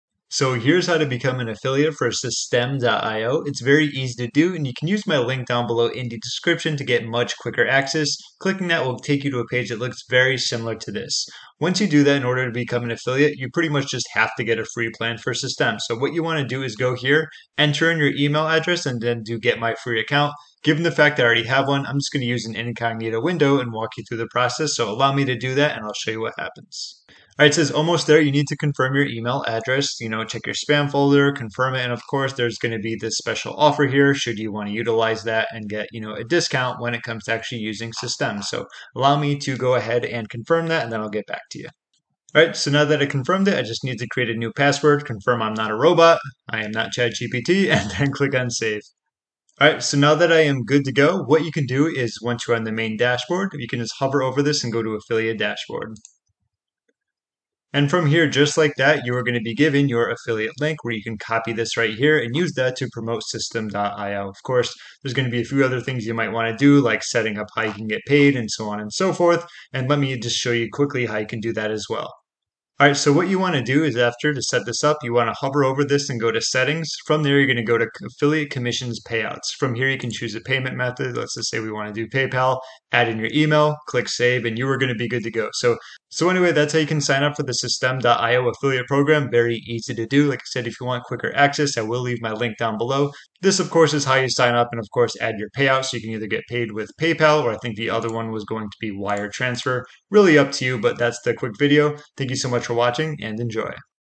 How To Become An Affiliate For Systeme.io (Affiliate Program Sign Up)
How To Become An Affiliate For Systeme.io

👋 Hey there! Thanks for casting away to Marketing Island to uncover "How To Become An Affiliate For Systeme.io." Here's what I'll be charting for you in this voyage:

🏝️ Systeme.io Affiliate Program Sign Up - Step onto the sandy shores of opportunity as we explore the Systeme.io Affiliate Program. I'll guide you through the lush landscapes of signing up, showing you every nook and cranny where treasure may be hidden.

🔍 I hope this video is the compass that guides you to the treasure you seek!